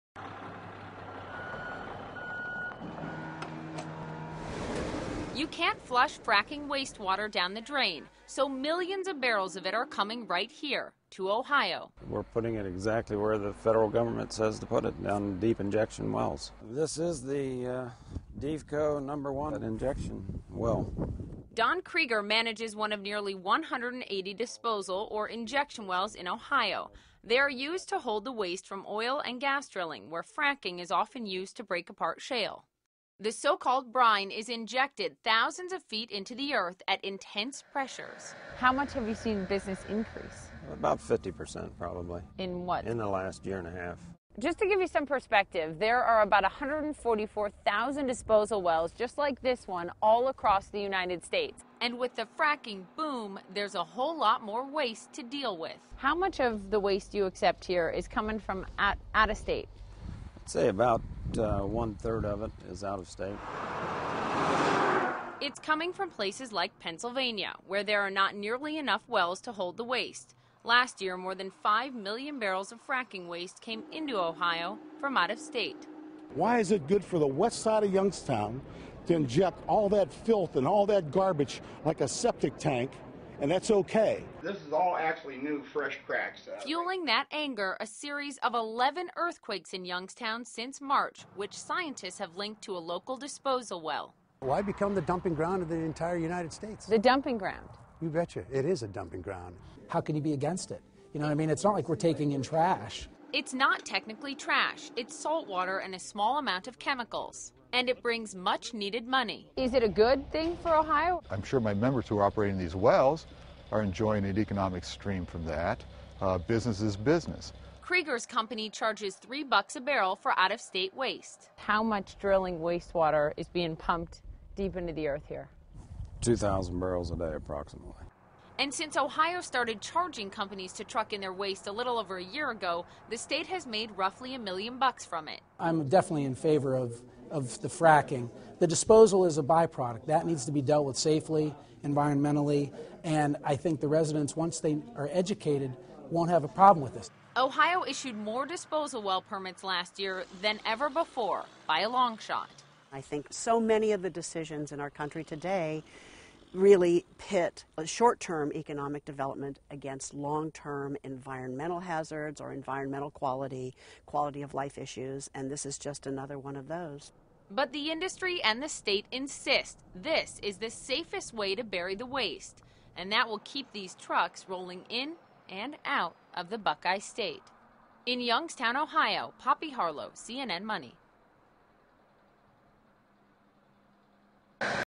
Ohio is importing large amounts of natural gas drilling wastewater from neighboring states
This reminds me of the South Park episode on the BP oil spill and the, i think, new logo BP took on of a picture of earth with a drill up its ass.